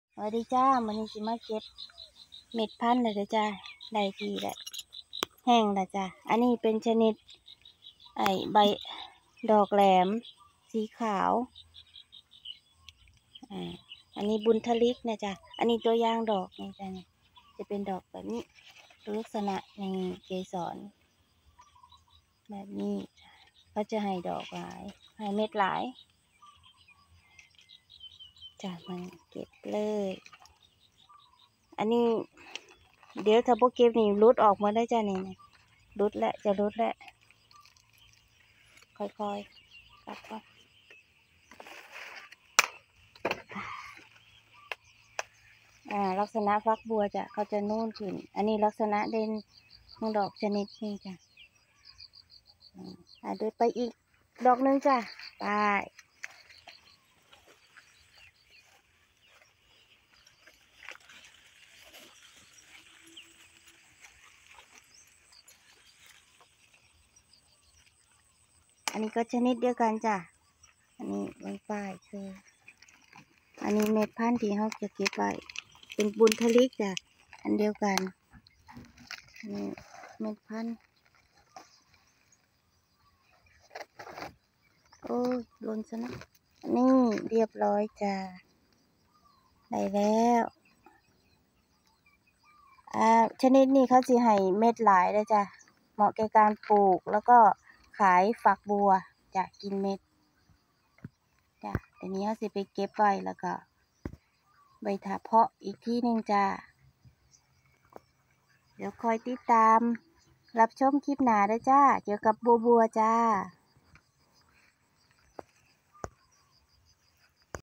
บัวหลวงบุณฑริก พันธุ์บัวหลวง
อยู่นาบัวเด้อจ้า เก็บเมล็ดพันธุ์บัวบุณฑริกเตรียมขยายพันธุ์ต่อ